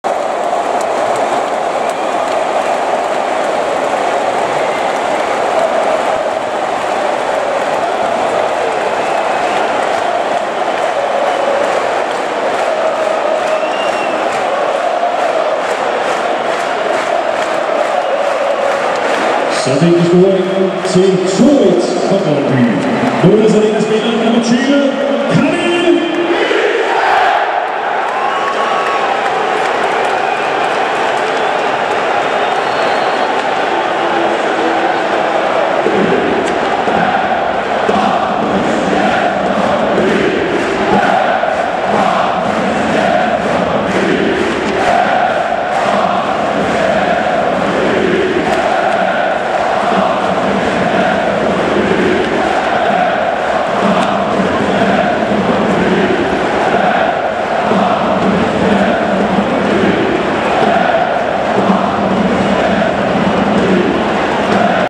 Fejring af 2 1 og hop nu hvis I Elsker Brøndby IF
Brøndbys fans under pokalfinalen 2018